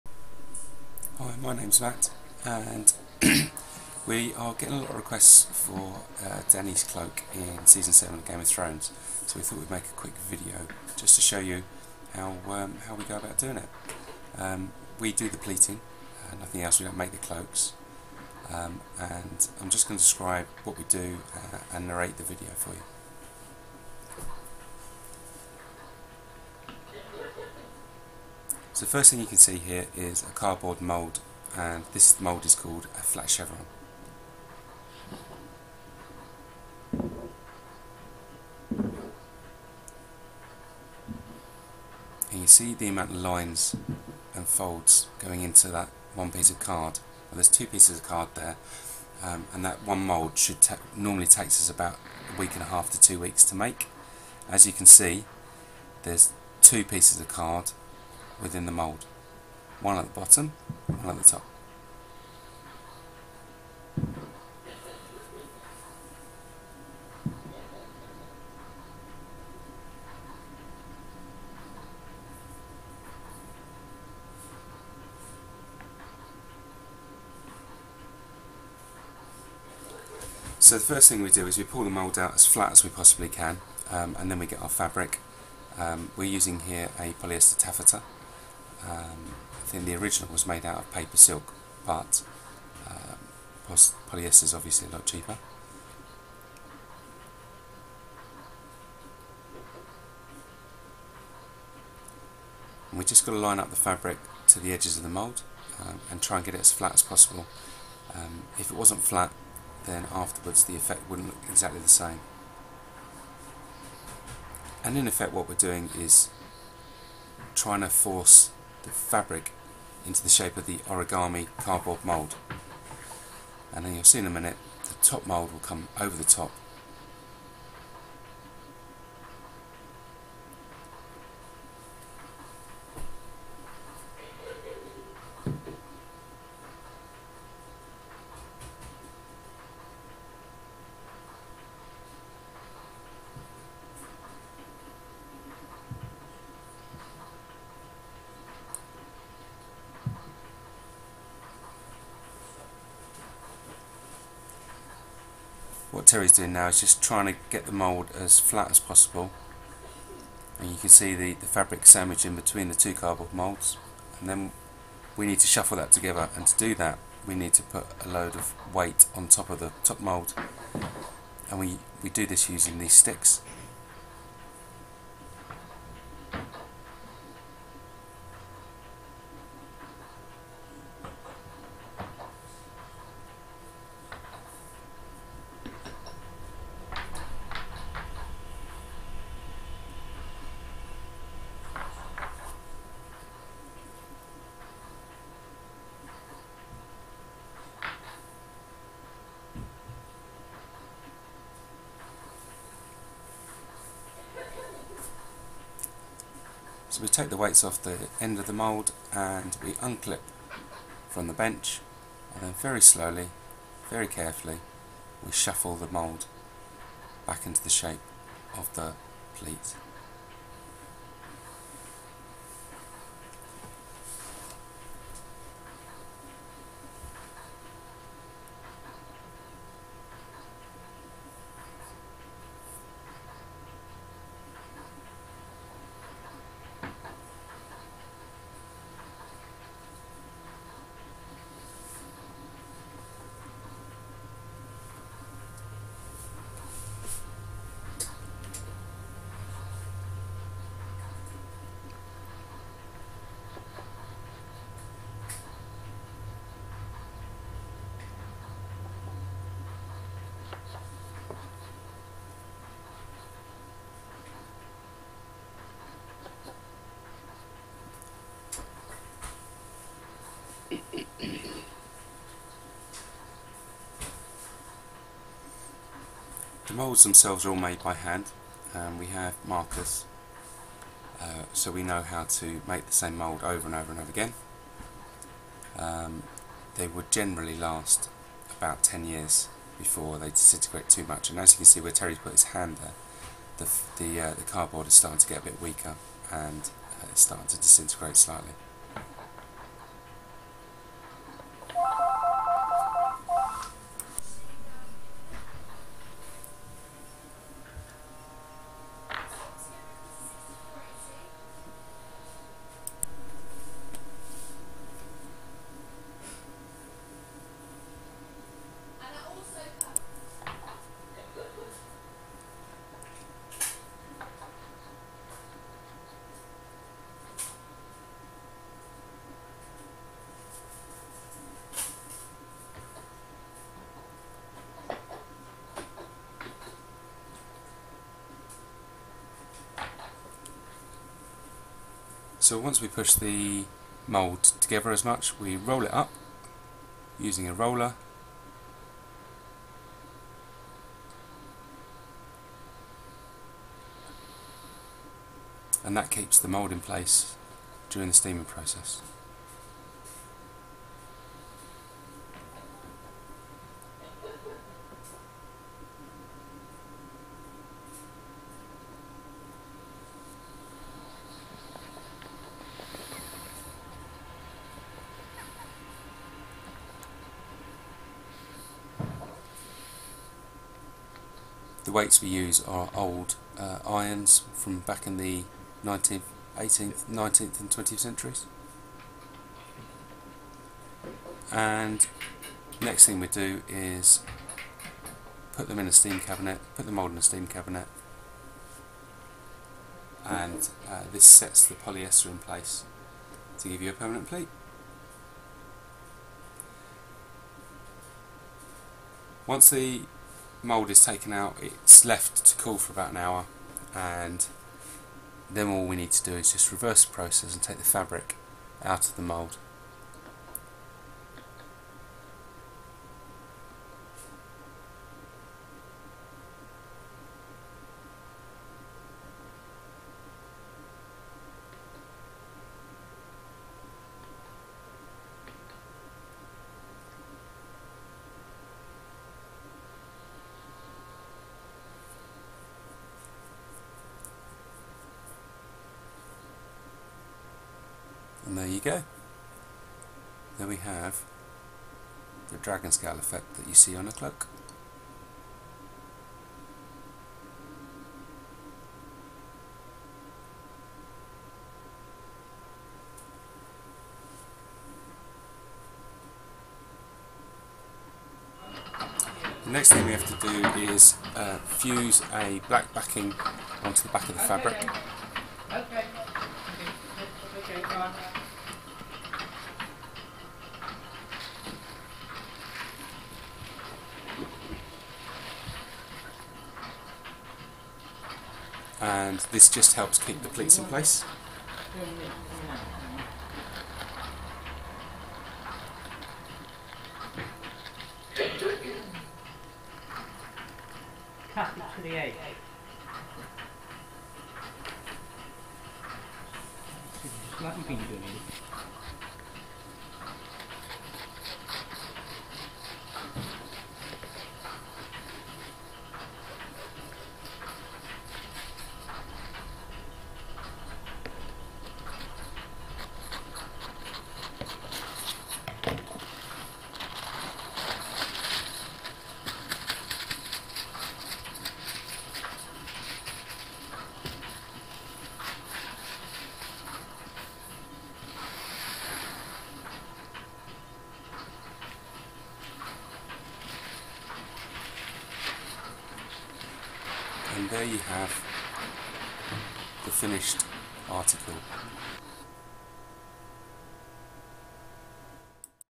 Pleating Daenerys Targaryen's Cloak from Season 7 GOT HD
How We pleated Dany's cloak from GOT season 7 in 720